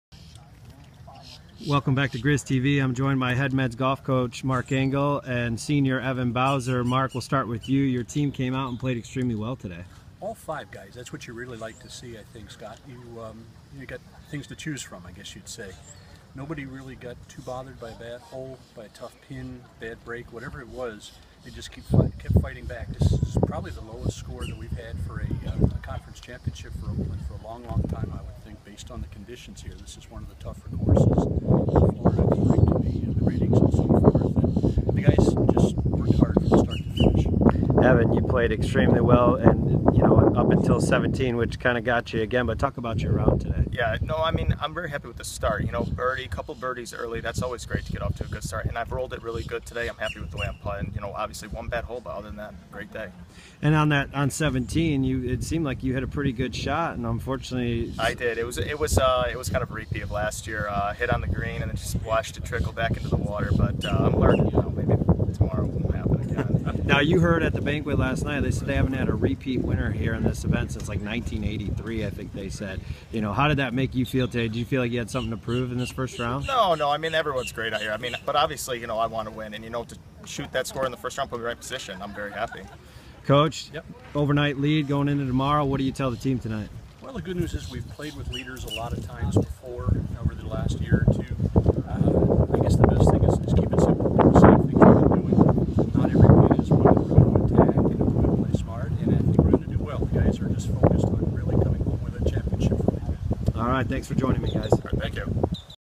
GrizzVision with Mark Engel and Evan Bowser 4/22/16
OaklandGolf men's coach Mark Engel and senior Evan Bowser talk about the Golden Grizzlies leading after day one of the Horizon League Men's Golf Championship.